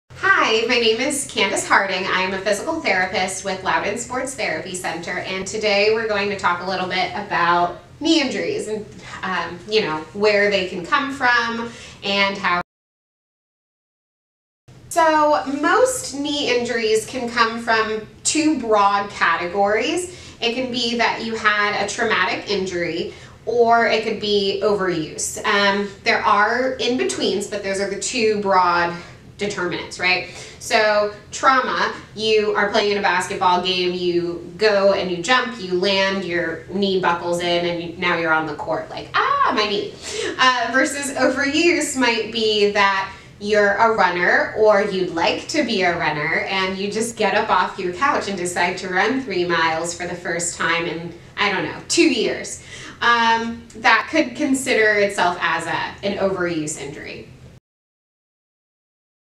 Broad Categories of Knee Injuries - Loudoun Sports Therapy Center
Knee injuries fall into two broad categories; Traumatic and Overuse. Both of these can be treated with the help of physical therapy!